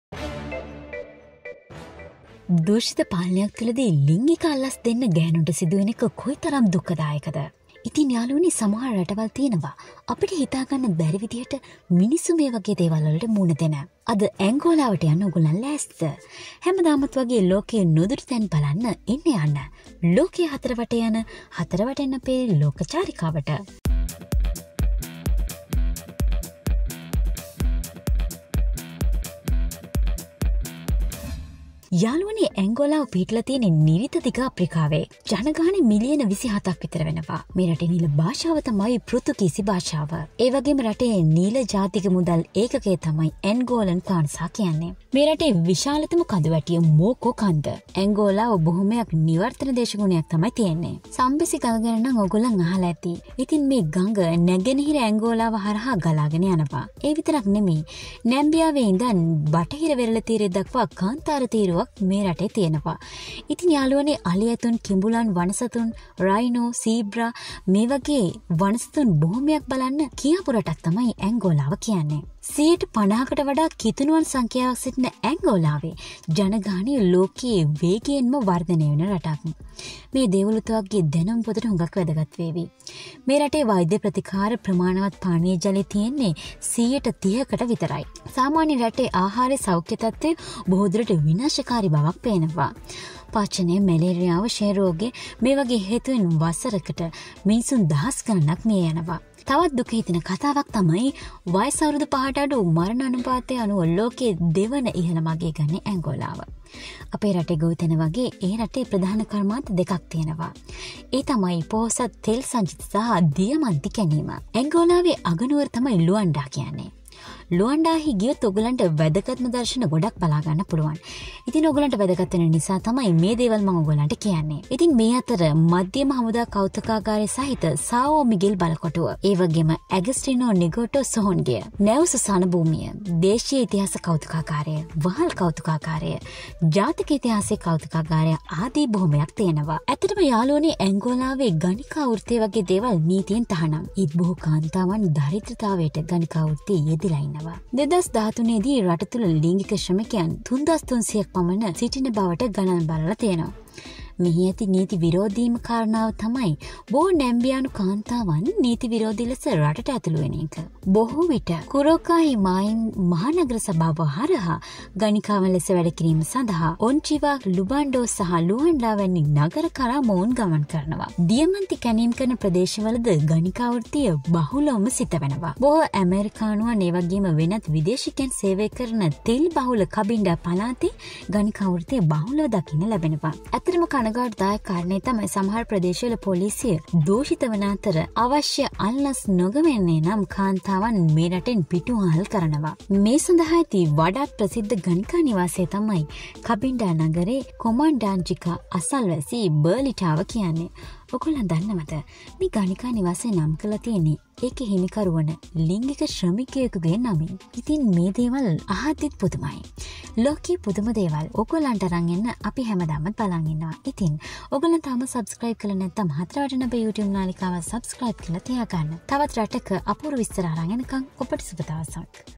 රටේ ගැහැණුන් පොලිසියට ලිංගික අල්ලස් දීලා ඉන්න තියෙන අමුතු රට | Hathara Waten
අද අපි මේ වීඩියෝ එකෙන් කතා කරන්න බලා බලාපොරොත්තු වෙන්නේ ඇන්ගෝලා රට පිළිබඳවයි, Angola  රටෙහි වර්තමාන තත්ත්වය හා එහි ජනජීවිතය පිළිබඳව මෙම වීඩියෝ එක මගින් අපි කතා කිරීමට බලාපොරොත්තු වෙනවා.